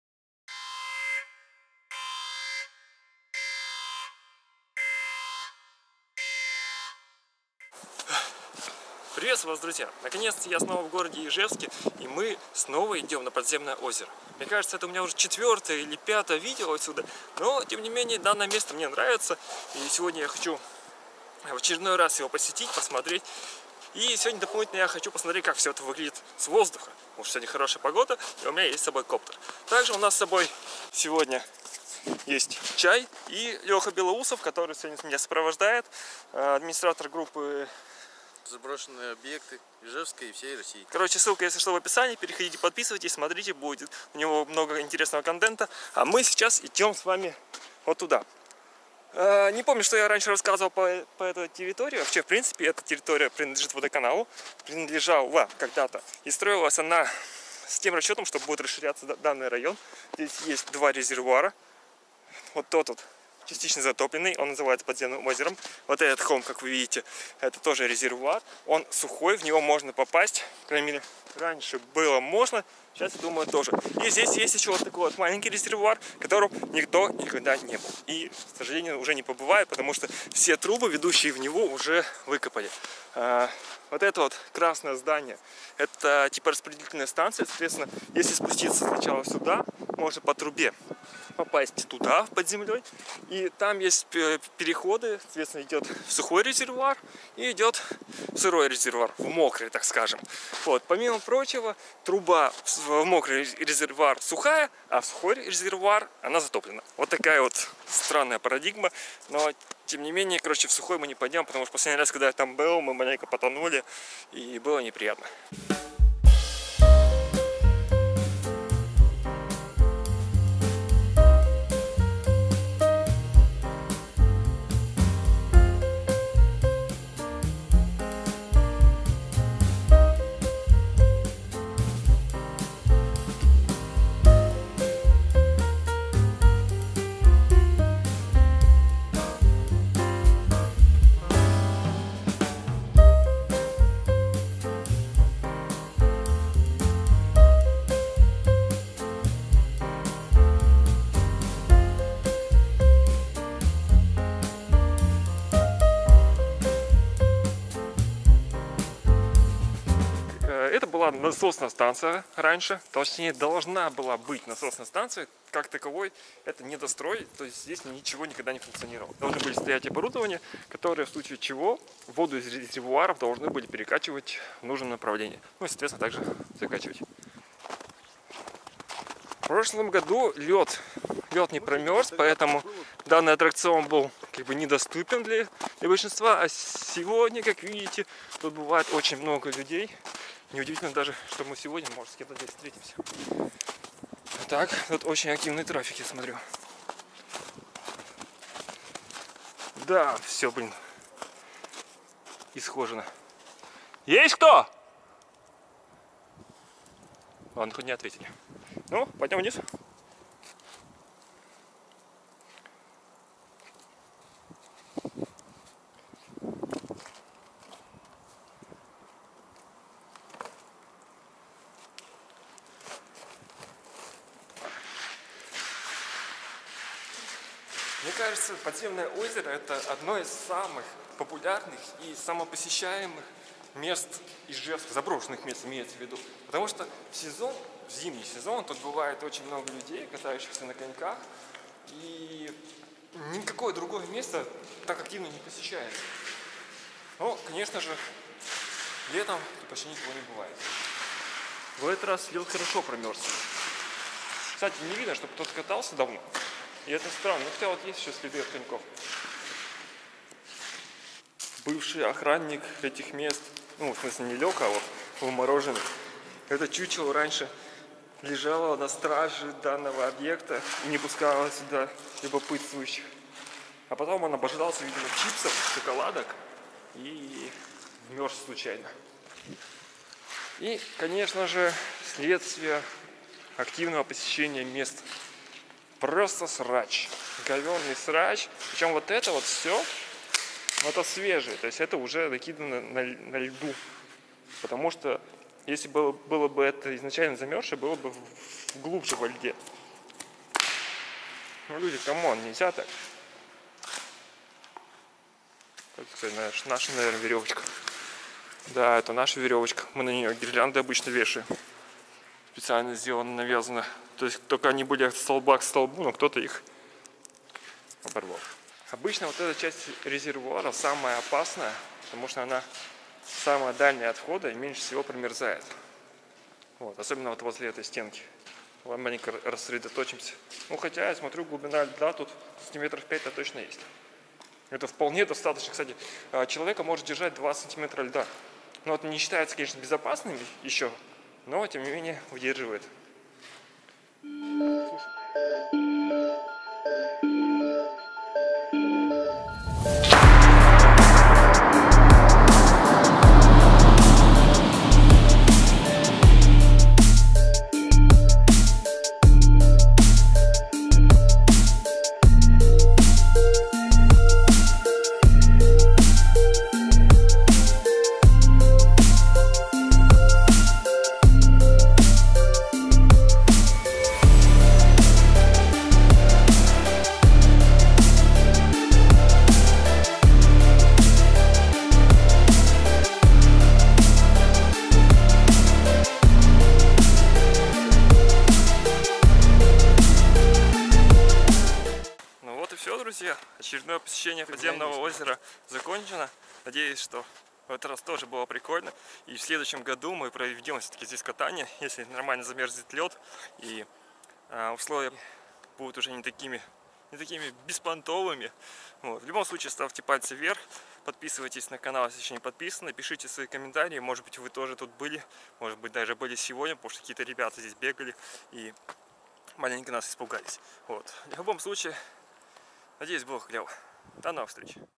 Ижевское подземное озеро \ Видео экскурсия и взгляд с воздуха
Технические резервуары которые превратились в заброшенное подземное озеро. Место становится очень популярным в зимний период, когда поверхность замерзает и превращается в настоящий каток.